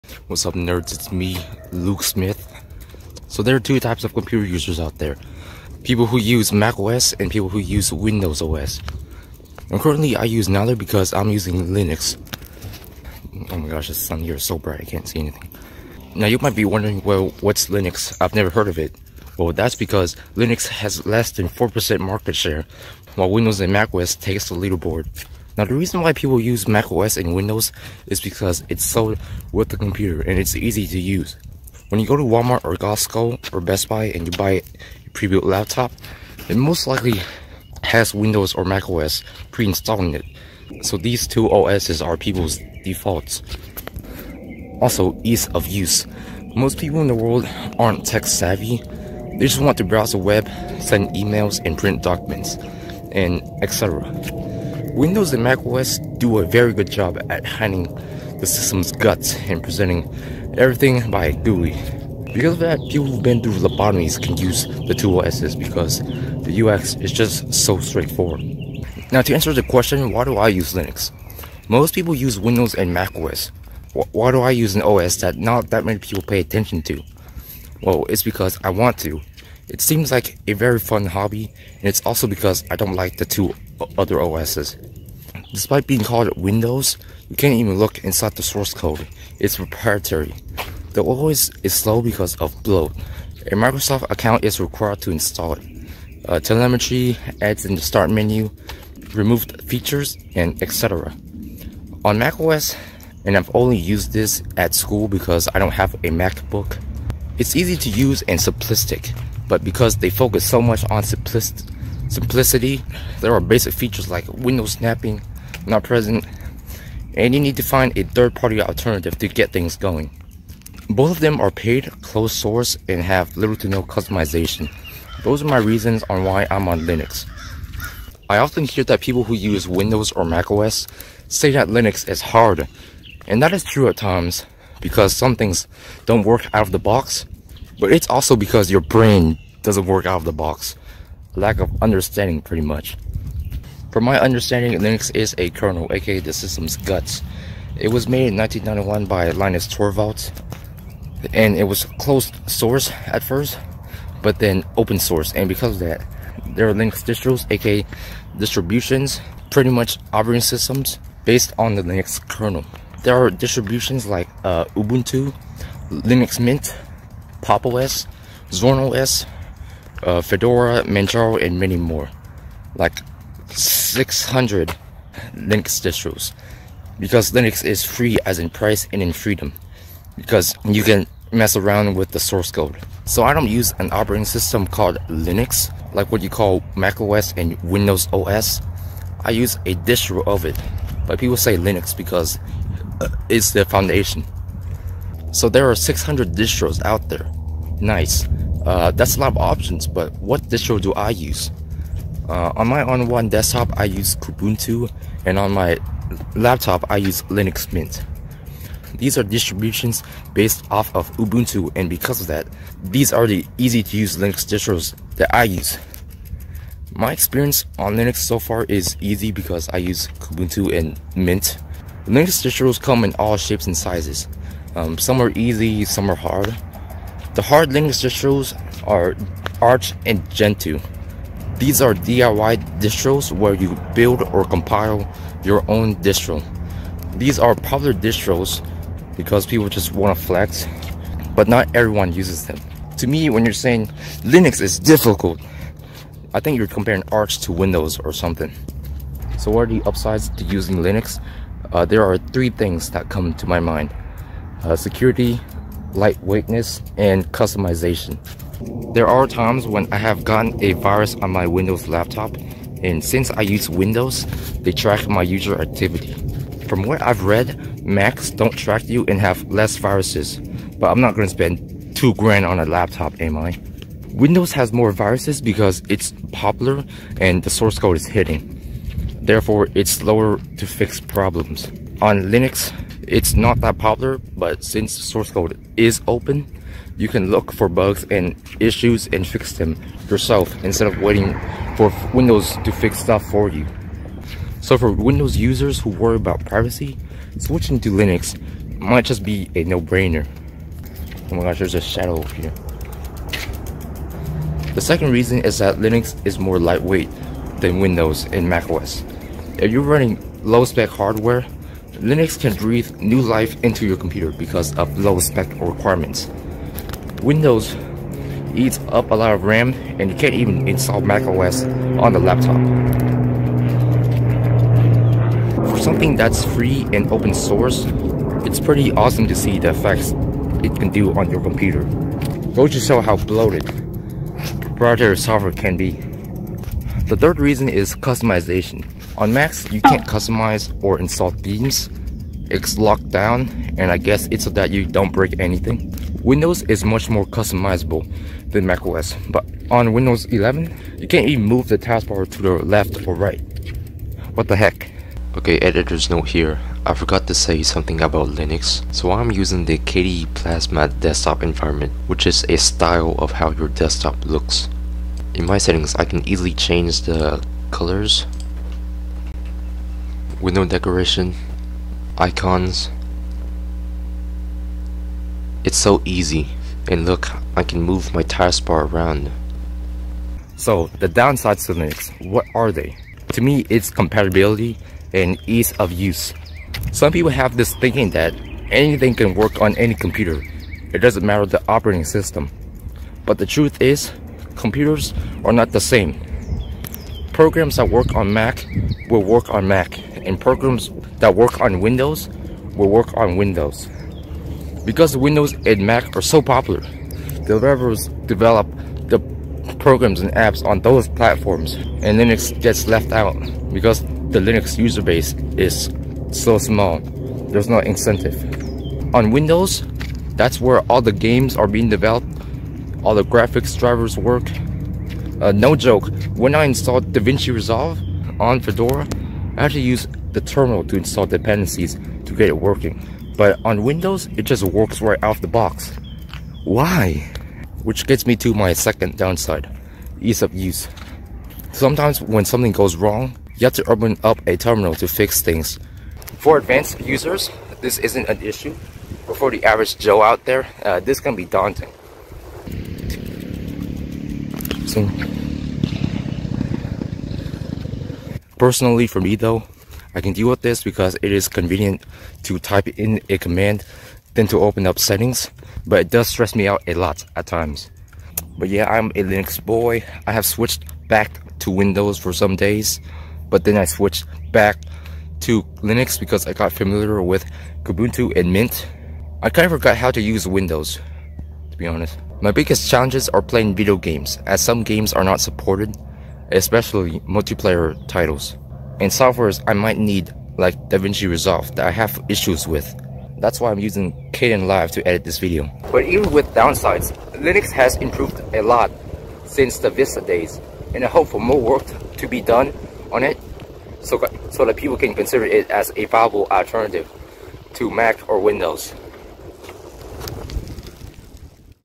Why I decide to use Linux
Most people use Windows or MacOS, but I am unique, I use Linux! This video is of my reasons why.
Linux has come a long way since the 2000s, it has gotten better and "easier" over the years and years to come, but it hasn't been adopted yet. Still, around 33 Million people use Linux and I can only hope that the number increases as time goes on...

0:03 - 2 Types of computer users
1:08 - Why do I use Linux?
2:23 - What is Linux?
3:15 - What Linux Distro did I choose?
4:09 - Upsides to using Linux
6:20 - Downsides to using Linux
7:45 - Linux Boii outro